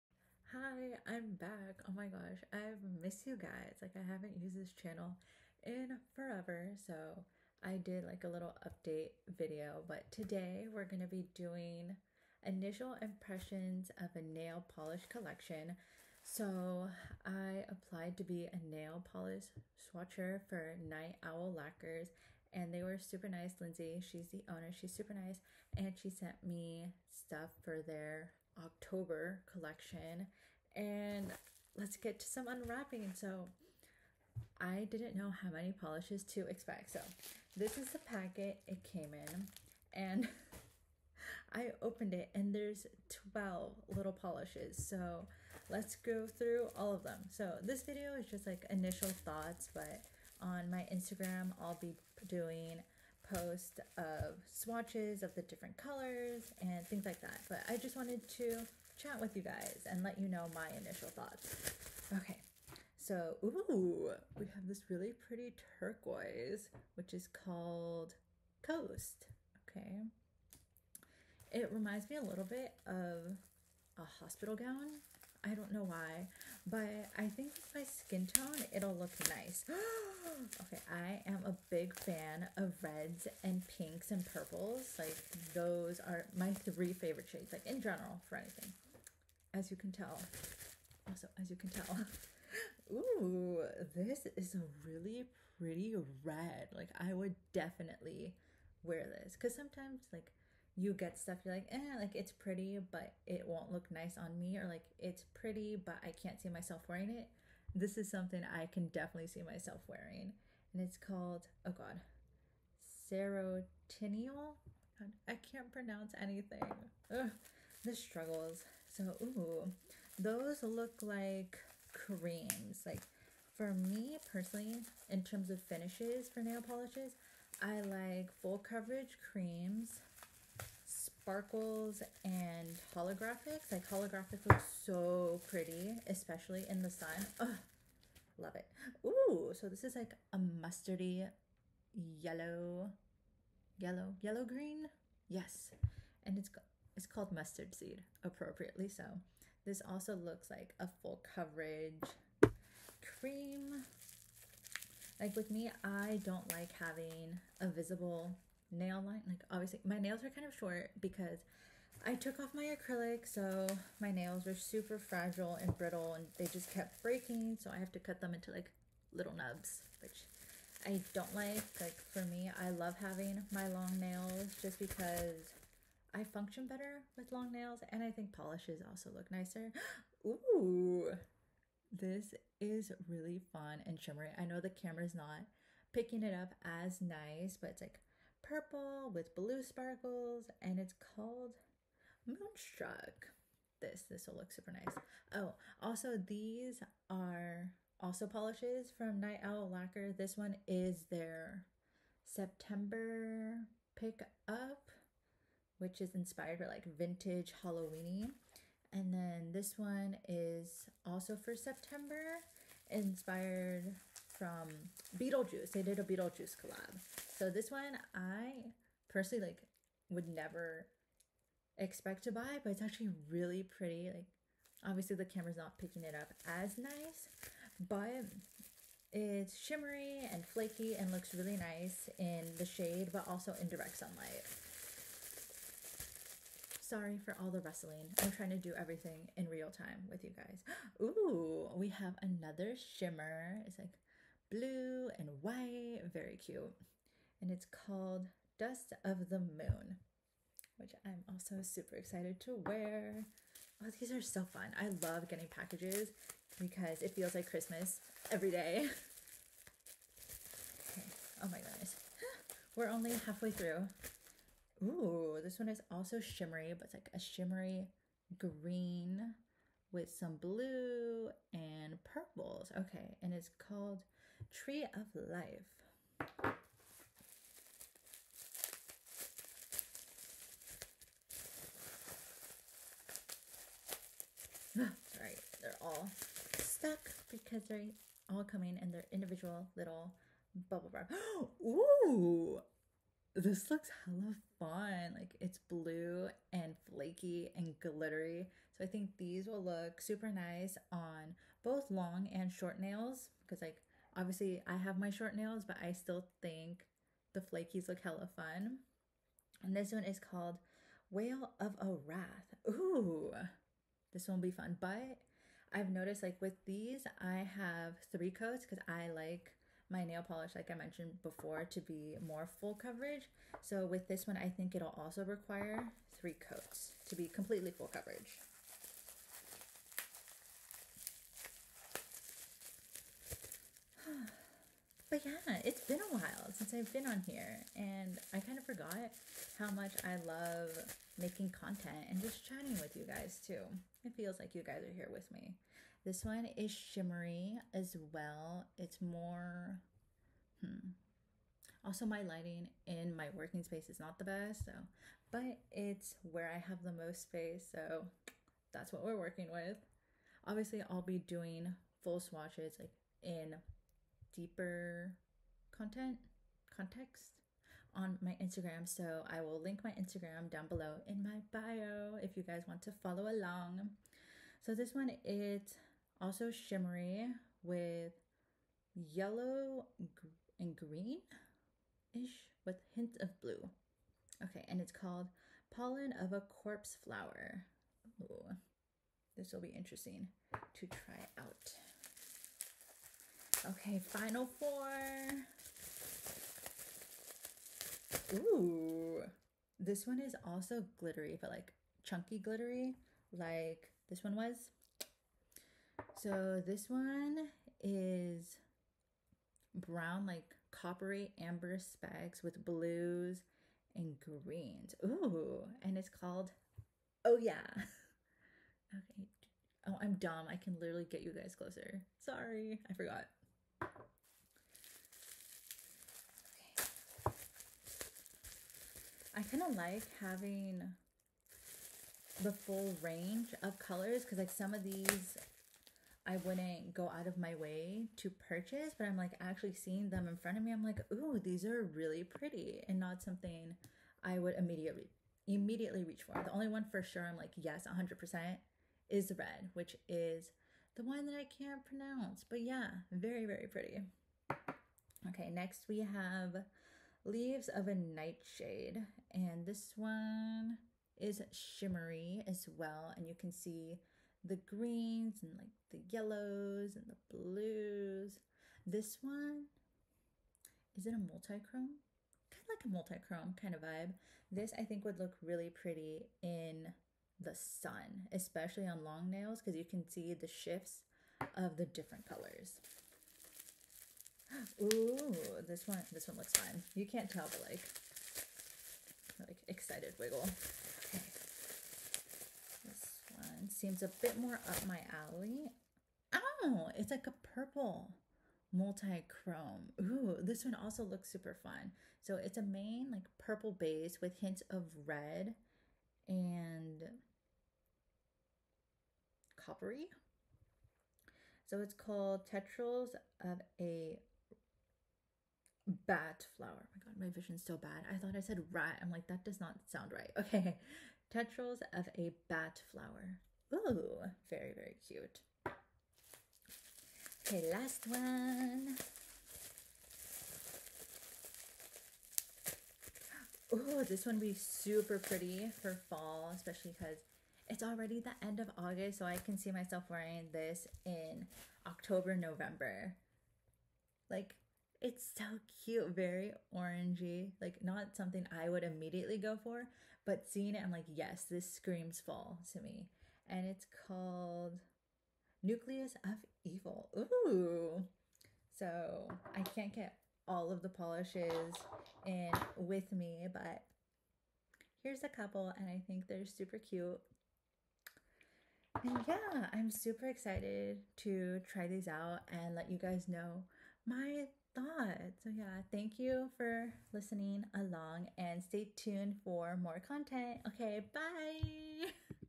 Night Owl Lacquer polish initial thoughts
Hiii I'm back with another nail polish unboxing.  After swatching these, I definitely liked some of these colors way more and I can't wait to show them off to y'all

Polish details:
July, August & September Creme of the Month
PNW Coast ($8) -- a teal blue creme
Serotinal ($8)-- a wine red creme (my personal favorite!)
Mustard Seed ($8)-- a brown toned mustard creme

Potion Ingredients Vol.4 Collection
The full collection will be priced at $70
Dust of the Moon ($13)-- a clear base filled with holographic dust and glowy shimmer that shifts aqua/blue/indigo.
Nucleus of Evil ($12)-- an orange jelly with orange/green/gold iridescent micro glitters and orange/copper/gold multichrome flakes.
Pollen of a Corpse Flower ($13)-- a clear base loaded with orange/gold/green shifting micro flakie shimmer, holographic dust & charcoal metallic micro flakies.
Leaves of a Nightshade ($13)-- a deep black based multichrome that shifts from bronze to multiple shades of green.
Tendrils of a Bat Flower ($13)--a deep black based multichrome that shifts blackened indigo/red/burgundy.
Wail of a Wraith ($12)-- a blackened blue jelly with blue/green/gold iridescent micro glitters and aqua/blue/violet multichrome flakes.

8 Year Anniversary Limited Edition
Moonstruck ($13)-- a violet tinted linear holographic base filled with burgundy/green shifting micro flakies and glowy shimmer that shifts green/blue/fuchsia.
This will be stocked thru Nov 13th, after that it will only be available until all remaining bottles sell out.

October Polish Pickup
Tree of Life ($13)-- a linear holographic multichrome that shifts light mossy green to olive green to dark green

October PBG
Oh, Yeah! ($13)-- a clear base filled with bronze reflective glitter pigment, turquoise metallic flakes and multichrome flakes that shift orange/copper/gold